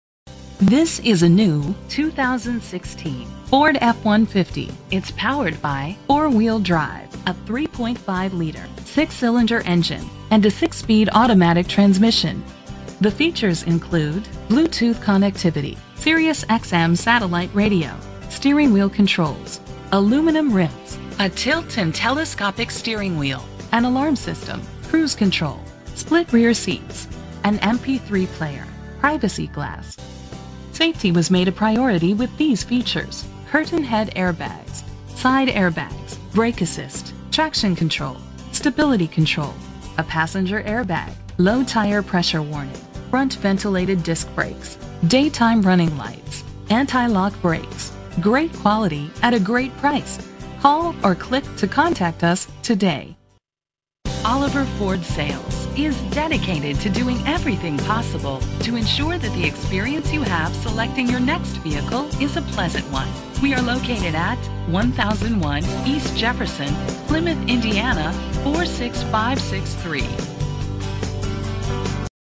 2016 Ford F-150 - Plymouth IN
Phone:
Year: 2016
Make: Ford
Model: F-150
Trim: XLT
Engine: 3.5 liter 6 cylinder 24 valve
Transmission: 6-Speed Automatic
Color: Race Red
Mileage: 54
Address:
1001 E. Jefferson
Plymouth, IN 46563